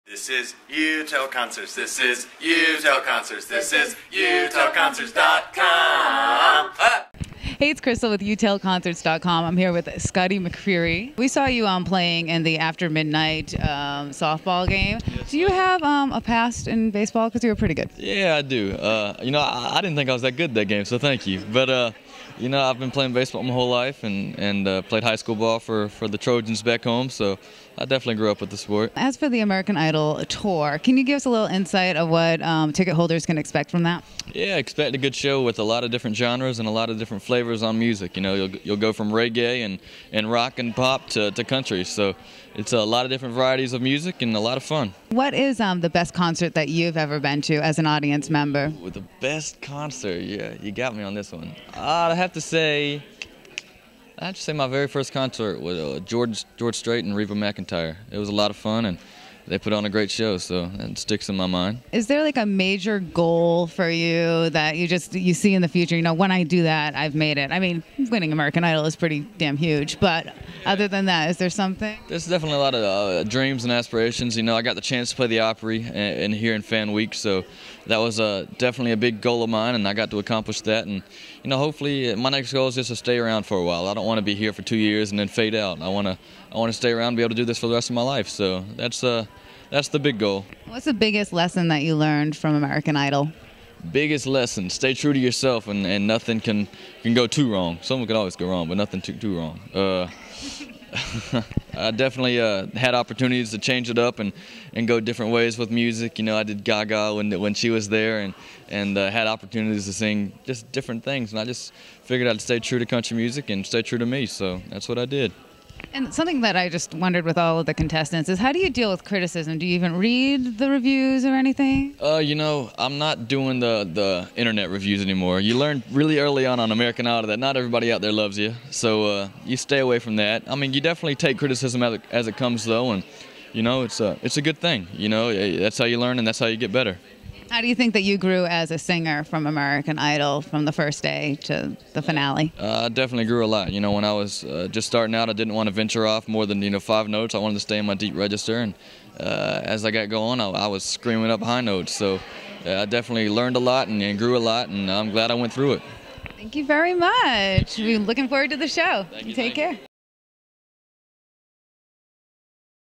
Interview w/ Scotty McCreery - American Idol Season 10 Winner
You Tell Concerts talks with American Idol winner, Scotty McCreery, currently on tour with American Idol Live. (Nashville, TN)